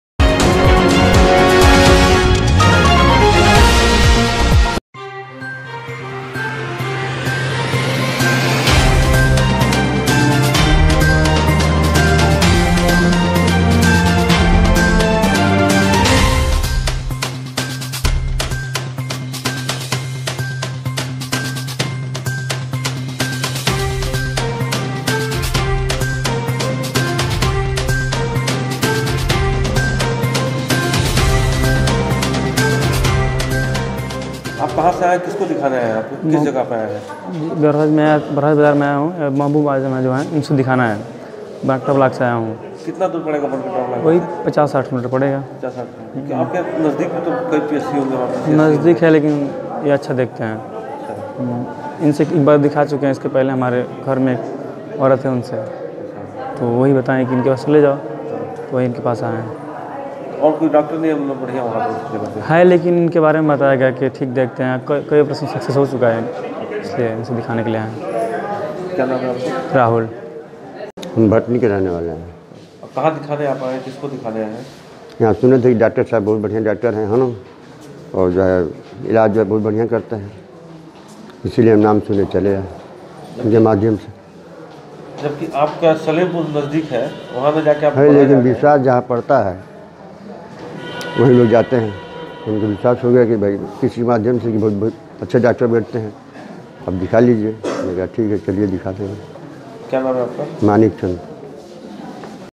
महबूब आजम बने मरीजो के नेत्रदाता
देवरिया जनपद के बरहज क्षेत्र सामूहिक स्वास्थ्य केन्द्र जहां पर तैनात आँख के  डॉक्टर महबूब आजम अपने कार्य को लेकर सुर्खियों में हैं उनके पास इलाज के लिये मरीजो की लाईन लगी रहती है ,और दूर दूर से मरीज अपना आंख  उनसे दिखाने के लिए आते है । और उनको संतुष्टि भी मिलती है ।मरीजो का कहना है की इनका यहाँ बहुत बढ़िया व्यवस्था है हम लोगो को काफी लाभ मिल रहा है।

वही प्रभारी निरीक्षक डॉक्टर महबूब आजम को इनके द्वारा किये गये आठ हजार आंखों के सफल आपरेशन करने के लिये पूर्व के मुख्यमंत्री ने इनको सम्मानित किया ।